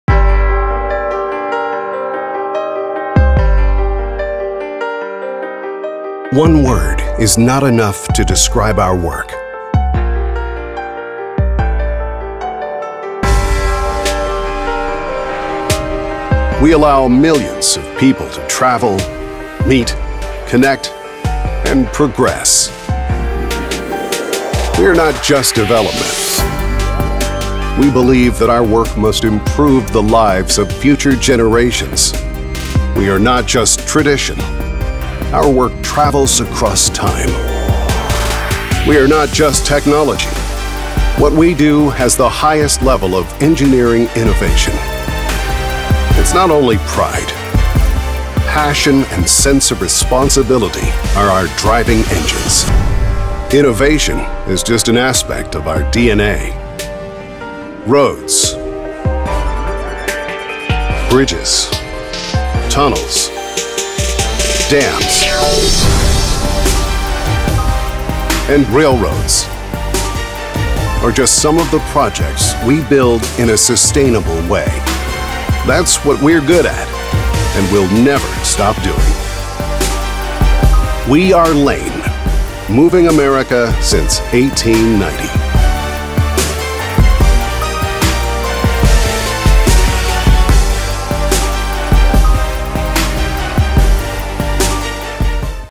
We Are Lane
Roads, bridges, tunnels, dams, and railroads are just some of the projects that we build in a sustainable way. That’s what we’re good at, and we’ll never stop doing it. We are Lane. Moving America since 1890.